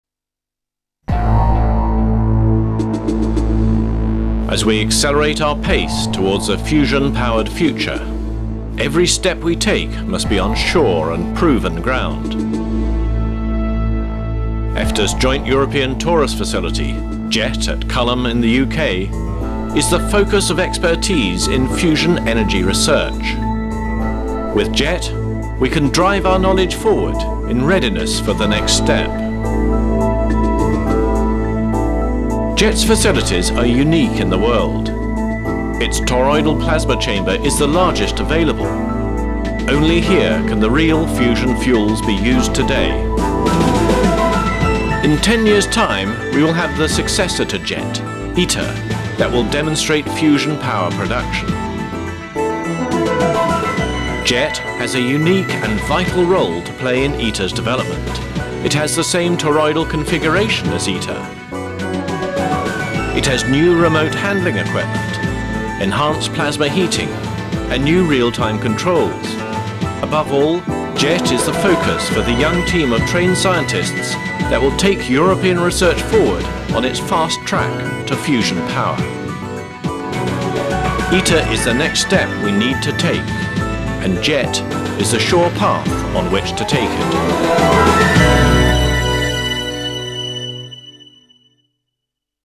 JET, the focus point of expertise
The movie provides you with a brief introduction to understand the vital role of JET as a focus point of expertise in fusion research.